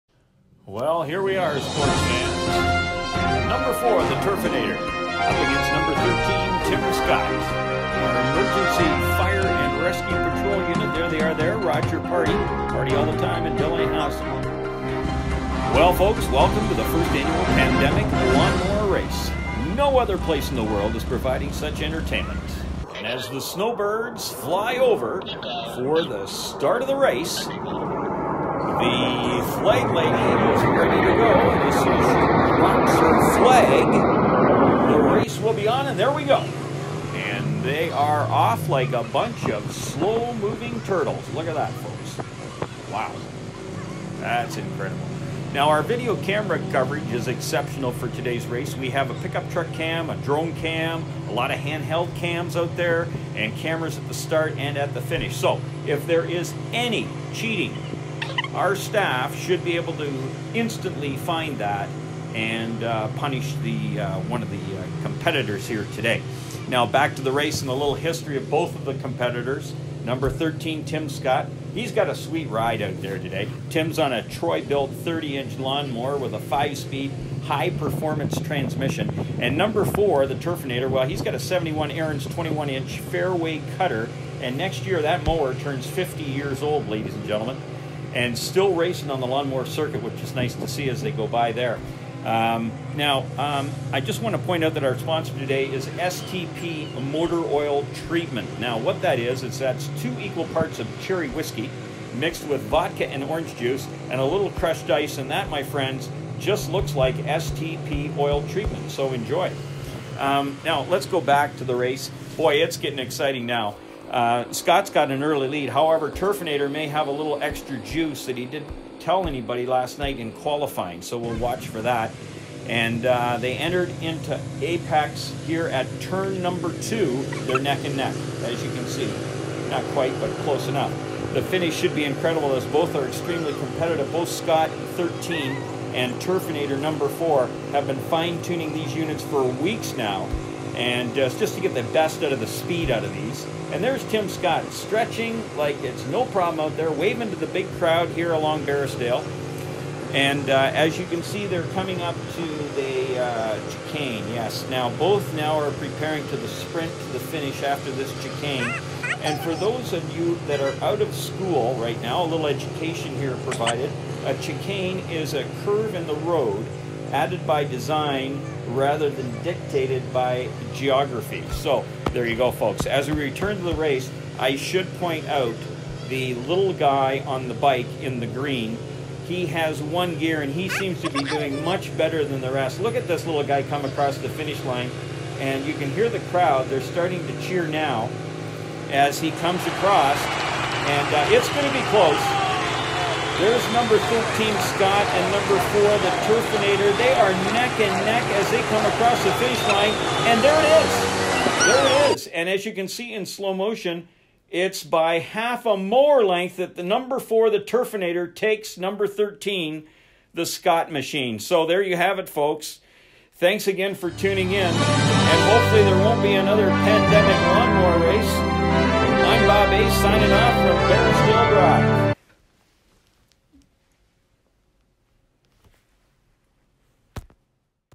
Covid-19 Lawnmower Race
Socially distanced sports fun on Barrisdale Dr. in Mississauga.